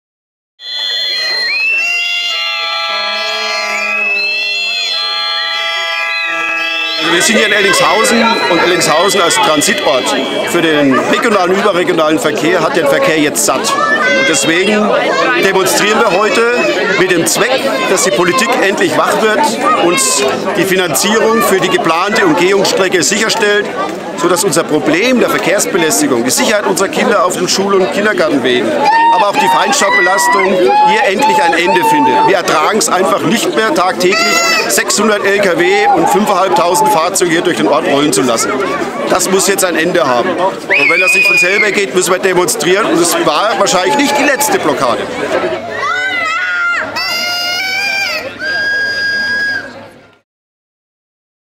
Eltingshausen: Traktoren riegeln Straße ab
Die Bürgerinitiative „Menschen statt Verkehr" will die neue Ortsumgehung für Eltingshausen und ist dafür auf die Straße gegangen.

Video: Benedikt Borst
Schnitt: Christian Bauriedel